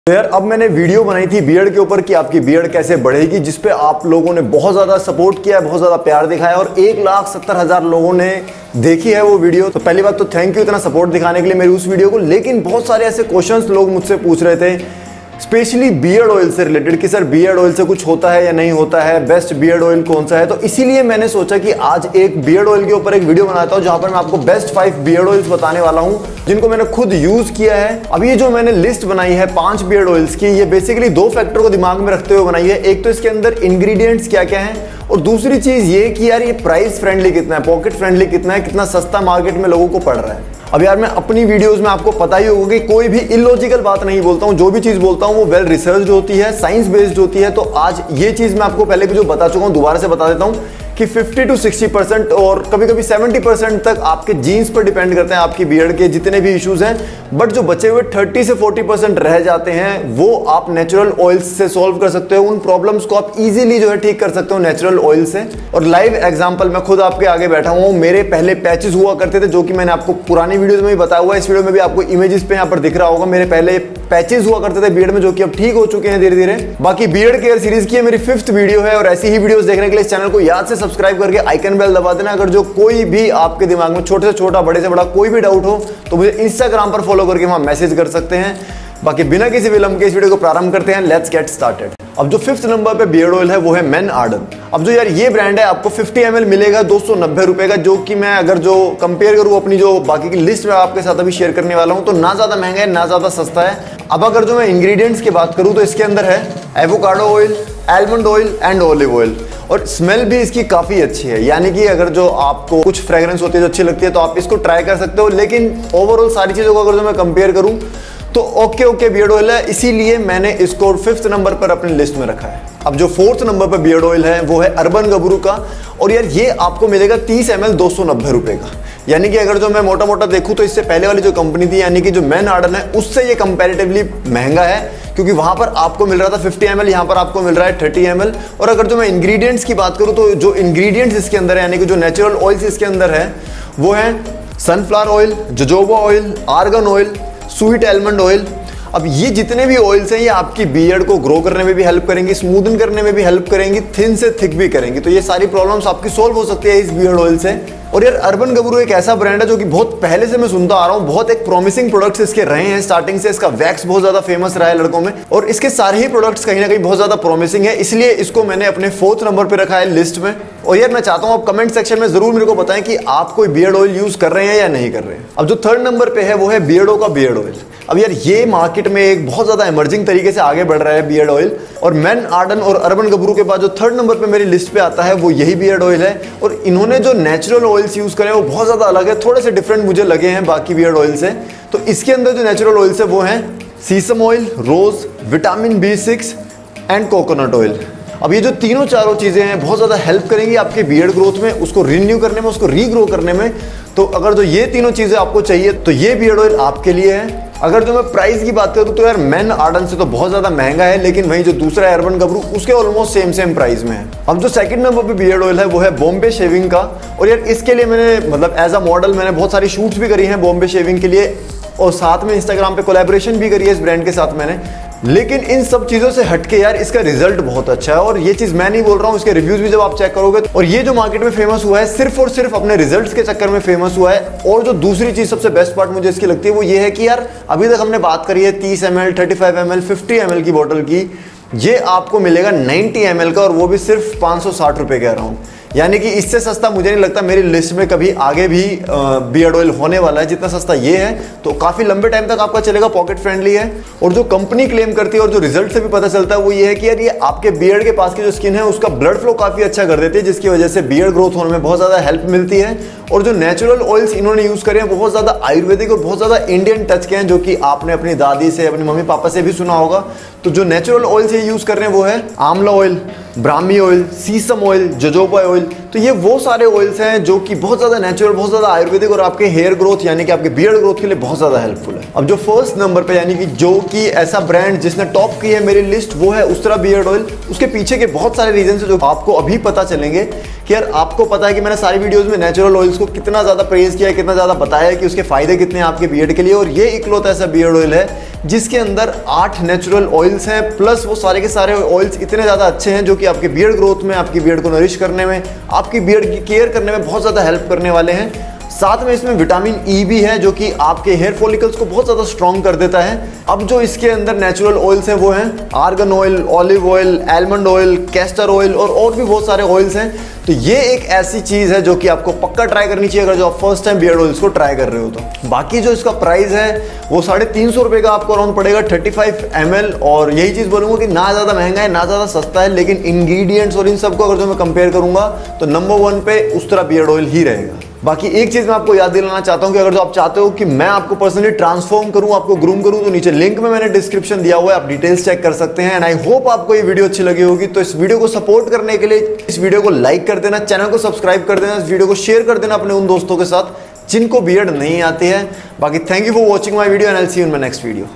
Top 5 Beard Oils In India 2020 in Hindi | Best Beard Oil For Beard Growth | By Puneet Tyagi
USTRAA BEARD GROWTH OIL

Top 5 Beard Oils In India 2020 in Hindi | Best Beard Oil For Beard Growth | By Puneet Tyagi  : In this video I have talked about 5 brands and ranked them from 5 to 1 in my list. this beard oil list is based on my personal experience. I have reviewed  these 5 brands , 1) ustraa beard oil review, 2) beardo beard oil review 3) urban gabru beard oil review 4) man arden beard oil review  5) bombay shaving beard oil review

My instagram username : Puneettyagiofficial